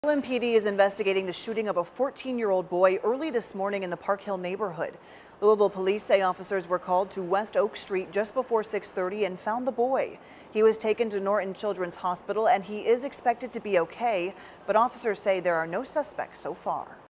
14-year-old injured in shooting on W. Oak Street
A 14-year-old boy was shot in the 2200 block of W. Oak St. around 6:25 a.m. October 7.